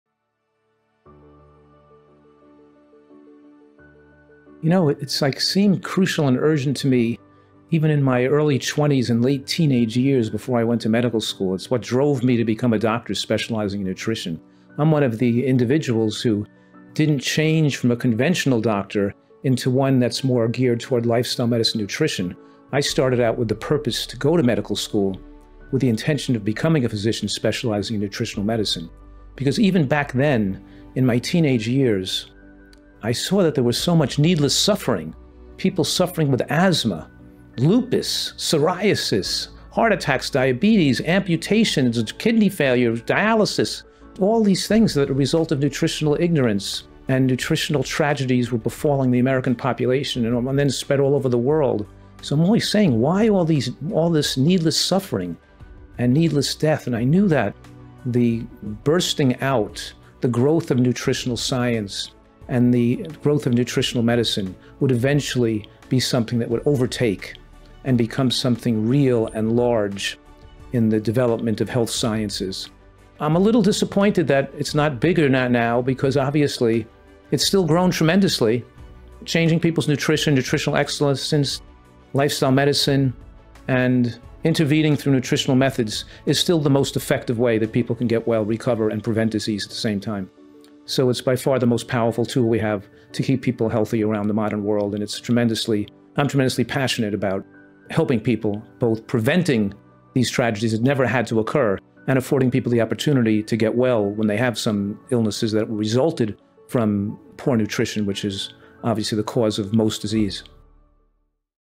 What Drove Me to Become a Doctor | Dr. Joel Fuhrman
Ever wondered why so many people suffer from health issues that could be easily prevented? Let Dr. Fuhrman introduce you to the world of the nutritarian diet along with nutritional medicine! Discover how simple dietary changes can transform your health, preventing and even reversing diseases like diabetes, heart disease, and more. 🍏🥦

In this eye-opening video, we explore the untapped power of nutrition in healthcare. From personal journeys to scientific insights, learn why nutrition is not just about eating right – it's about living right. Don't miss out on these life-changing tips and stories. Watch now and take the first step towards a healthier you!